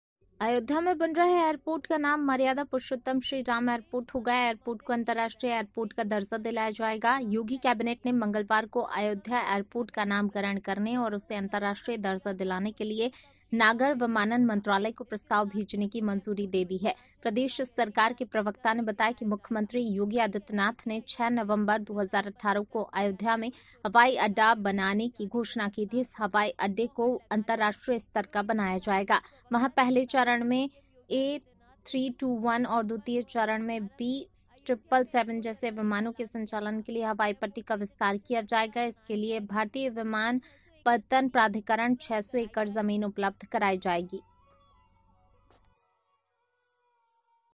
अयोध्या मे बन रहा मर्यादा पुरुषोत्तम श्रीराम एयरपोर्ट
अयोध्या में बन रहे एयरपोर्ट का नाम मर्यादा पुरुषोत्तम श्रीराम एयरपोर्ट होगा। एयरपोर्ट को अंतरराष्ट्रीय एयरपोर्ट का दर्जा दिलाया जाएगा। योगी कैबिनेट ने मंगलवार को अयोध्या एयरपोर्ट का नामकरण करने और उसे अंतरराष्ट्रीय दर्जा दिलाने के लिए नागर विमानन मंत्रालय को प्रस्ताव भेजने की मंजूरी दे दी।
प्रदेश सरकार के प्रवक्ता ने बताया कि मुख्यमंत्री योगी आदित्यनाथ ने 6 नवंबर 2018 को अयोध्या में हवाई अड्डा बनाने की घोषणा की थी। इस हवाई अड्डे को अंतरराष्ट्रीय स्तर का बनाया जाएगा। वहां पहले चरण में ए-321 और द्वितीय चरण में बी-777 जैसे विमानों के संचालन के लिए हवाई पट्टी का विस्तार किया जाएगा। इसके लिए भारतीय विमानपत्तन प्राधिकरण 600 एकड़ जमीन उपलब्ध कराई जाएगी।